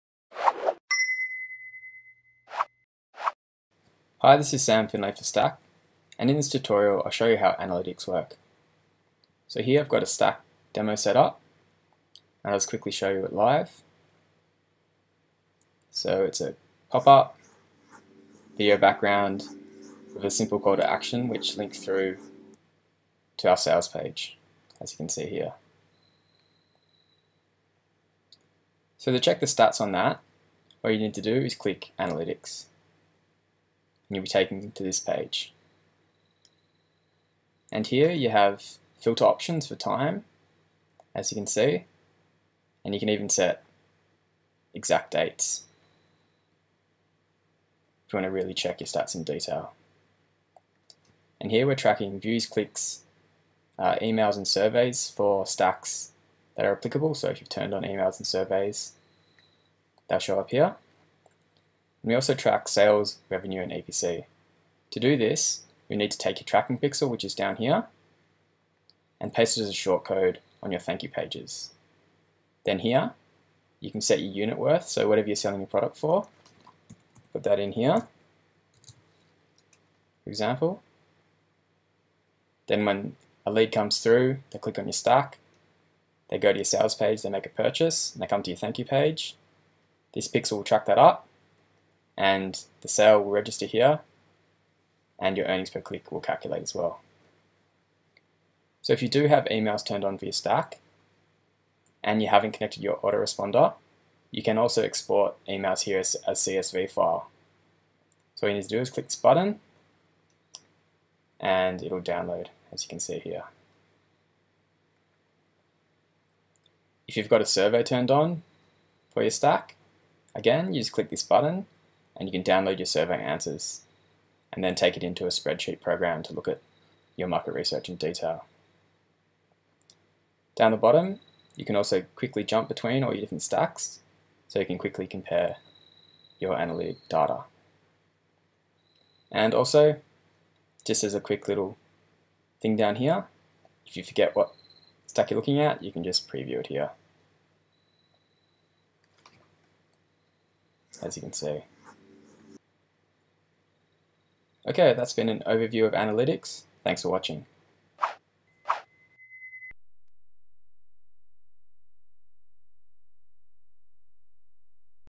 Accessing Analytics
In this video I show how Analytics work in Stakk and how to easily track your sales, EPC and export your email captures and market research data.